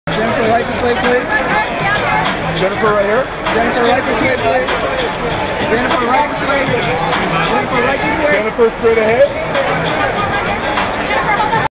Jennifer Getzinger at 65th Annual Directors Guild Of Amer...
Jennifer Getzinger at 65th Annual Directors Guild Of America Awards - Arrivals 2/2/2013 in Hollywood, CA.